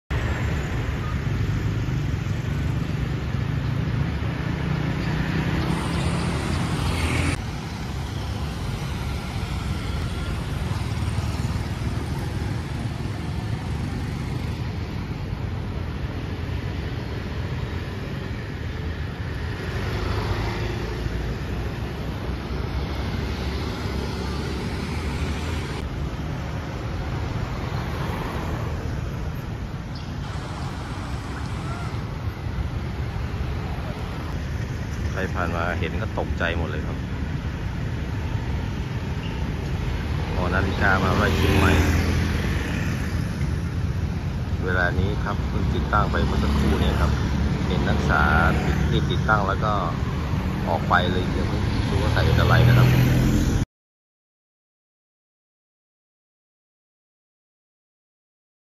นศ.มช.แสดงออก 6 ต.ค.ทำหุ่นแต่งชุดนักศึกษาเปื้อนเลือดมัดติดหอนาฬิกา มช.
6 ต.ค.2565 นศ.มช.ได้นำหุ่นแต่งกายชุดนักศึกษา (นศ.) นำมามัดติดไว้กลางเสาหอฬิกา มหาวิทยาลัยเชียงใหม่ (มช.)